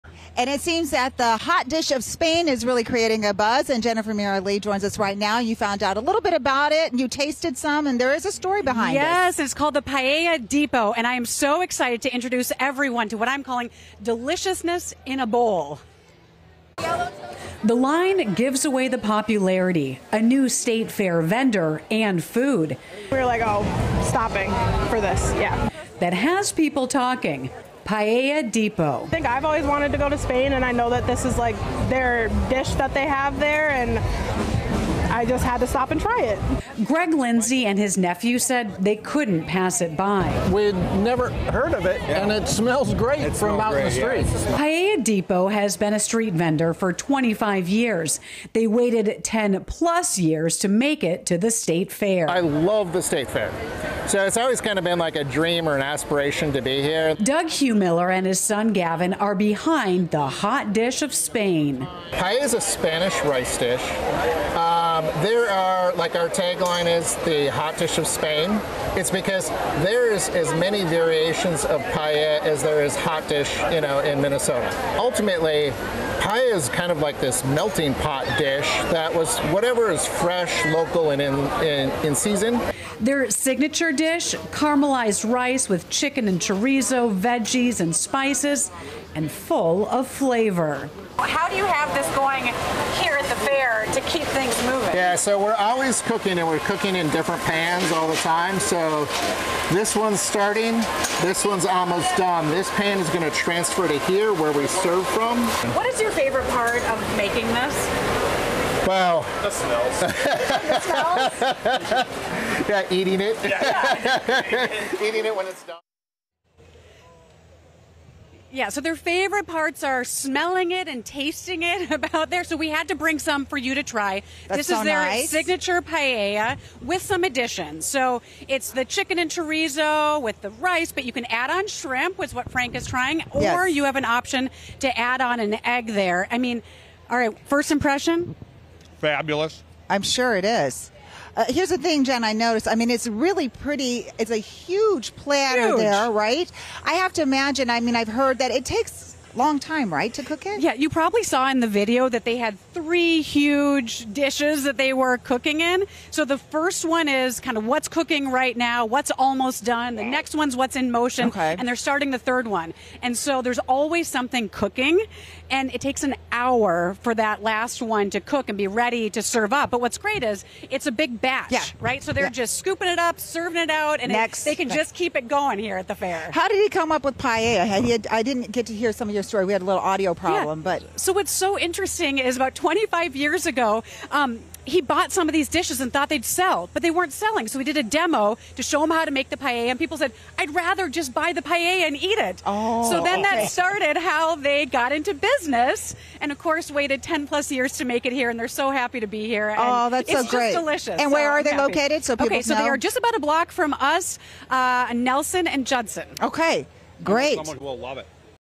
A new vendor is bringing some Spanish flare to the Minnesota State Fair
As WCCO's Jen Mayerle shows us, the Paella Depot is becoming a quick favorite at the Minnesota State Fair this year.